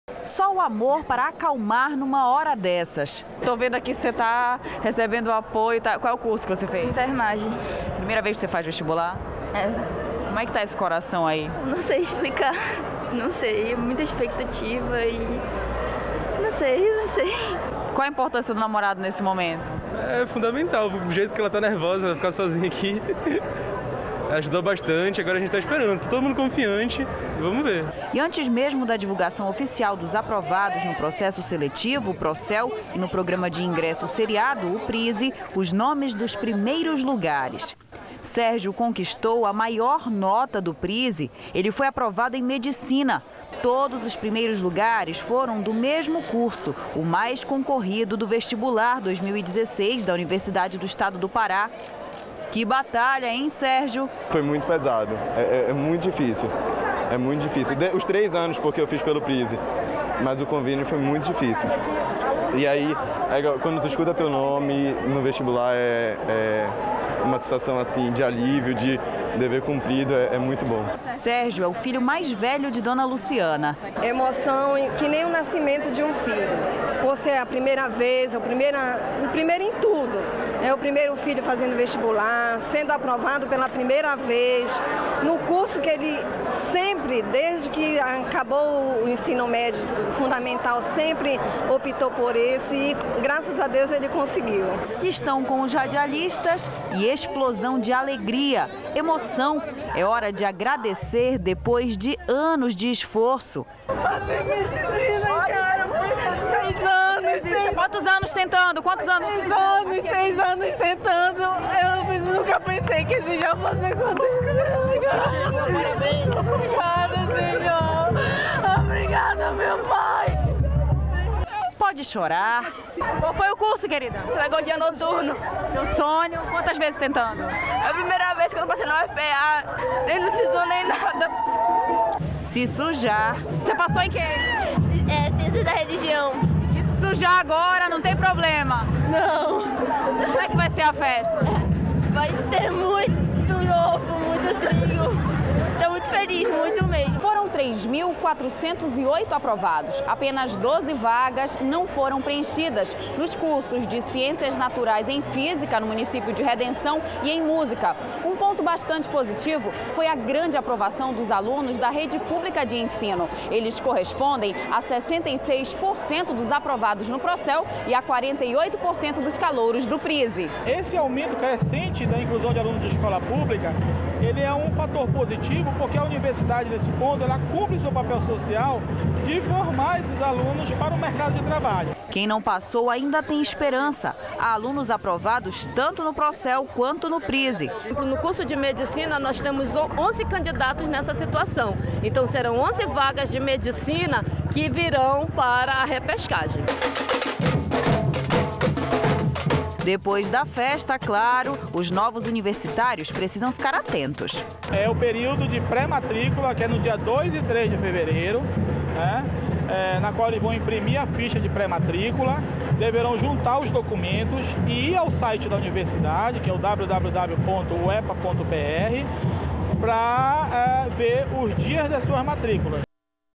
SBT Pará (28/01/2016)_Listão Vestibular UEPA
Hoje é dia de festa para os novos calouros da Universidade do Estado do Pará.
3.408 estudantes passaram nos processos seletivos Prosel e Prise.
Agora é ficar atento ao prazo de matrícula. O site da universidade é